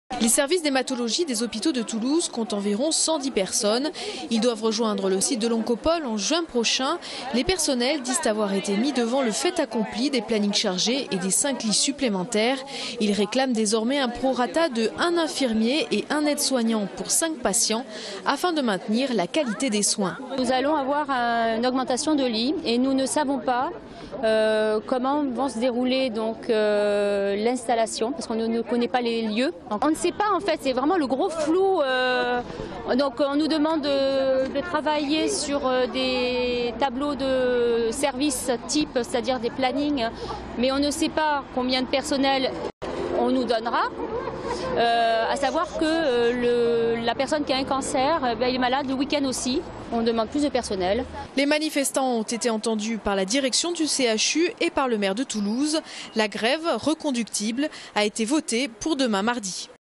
Santé : Grève du service d'hématologie (Toulouse)
Sites webs/blogs, copiez cette vidéo avec le bouton "Intégrer" !
 Coup de projecteur sur la grève des infirmiers et des aides-soignants du service d'hématologie du CHU de Toulouse.